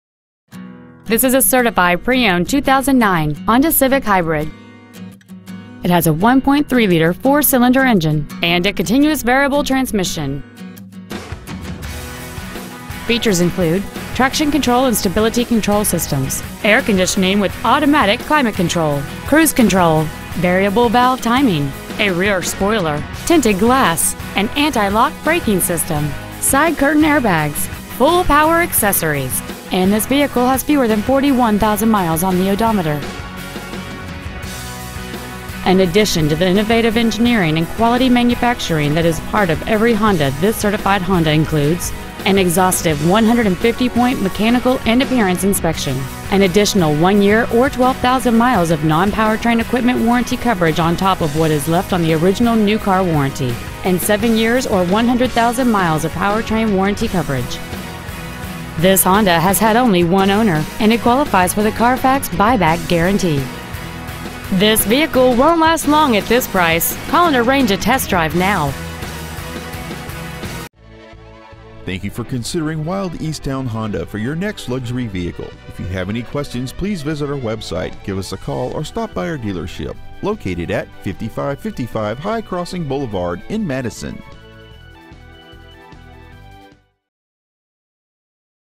Used 2009 Honda Civic Hybrid Monona WI
We have been honored to serve the Madison WI area , we promise that your experience at our dealership will exceed your expectations !

 Year : 2009
 Make : Honda
 Model : Civic Hybrid
 Engine : 1.3L 4 cyls Hybrid
 Trans . : CVT (Continuously Variable)
 Exterior : Black
 Miles : 40,956

 Stock : H1168

 5555 High Crossing Blvd.